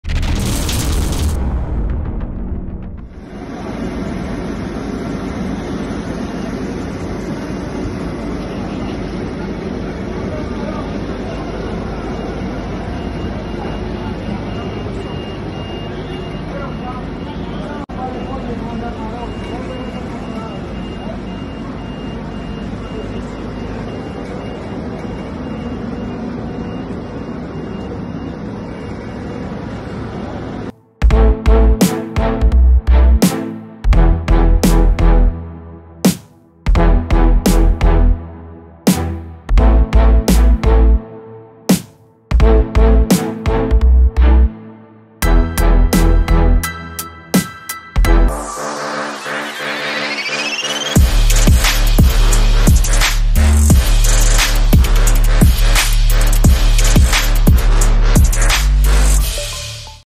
Conor Mcgregor Recieves His Custom Lambo Super Yacht
Mcgregor unloads his new custom Lamborghini Tecnomar 63 super yacht. .

1)This video has no negative impact.
2)This video is also for teaching & Informational AND Entertainment purposes.
3)It is not transformative in nature.